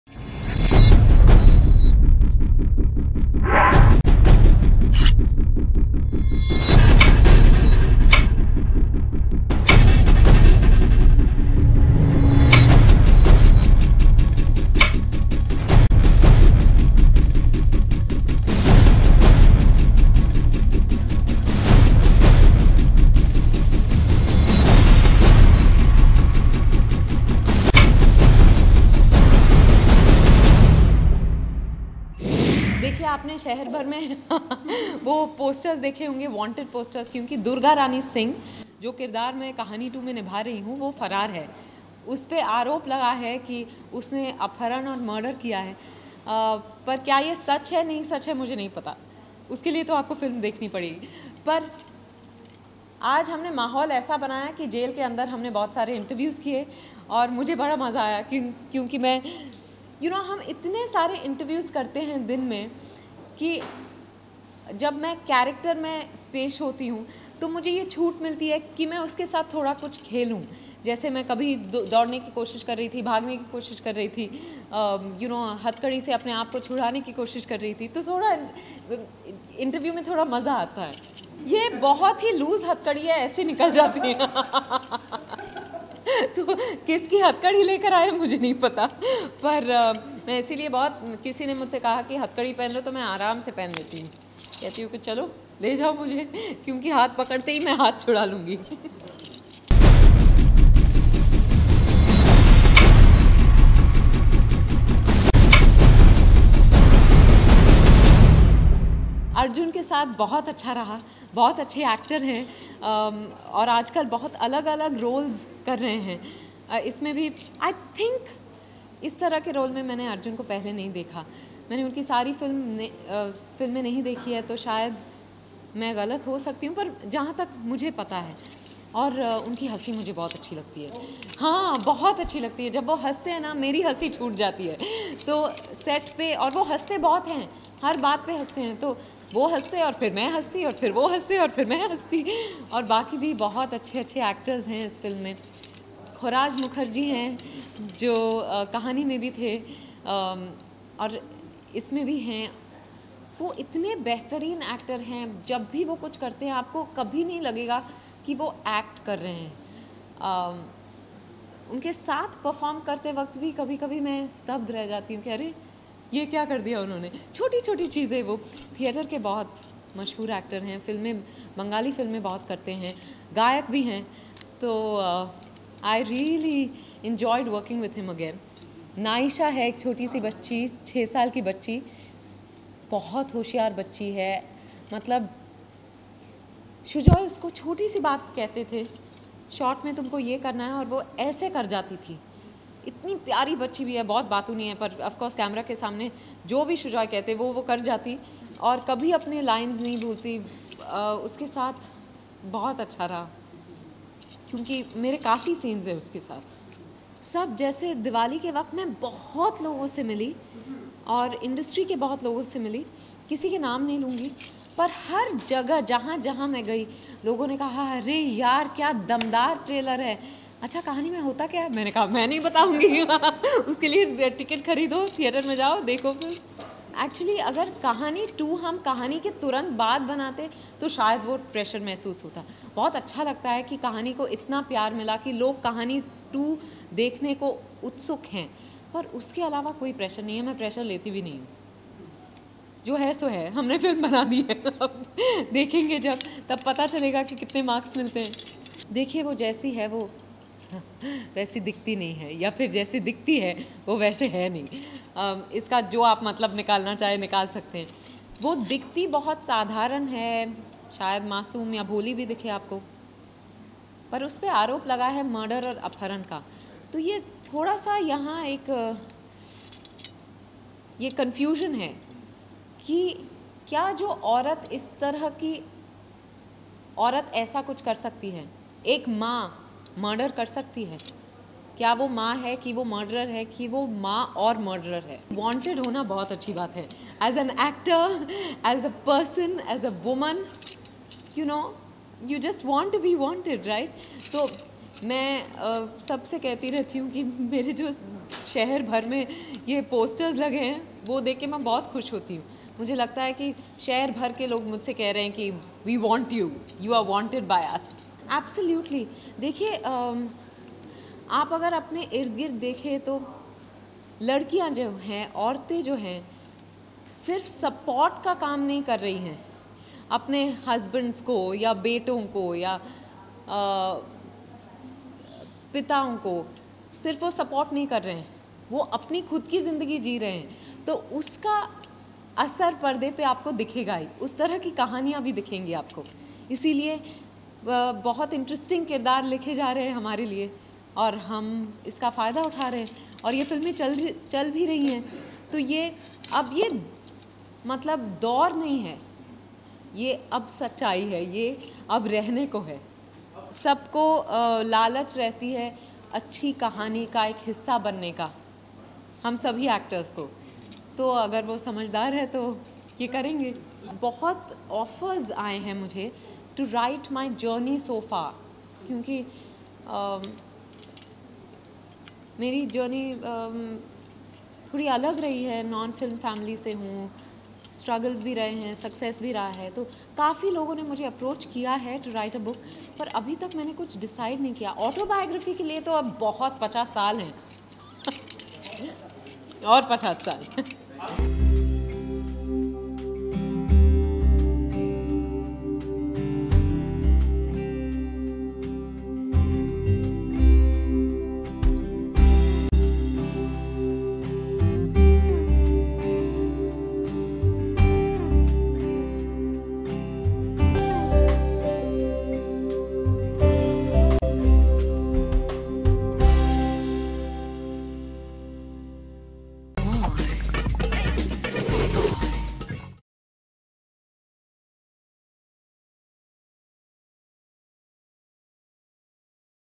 Vidya Balan goes behind Bars to promote 'Kahaani 2' | SpotboyE
Vidya Balan goes behind Bars to promote 'Kahaani 2'.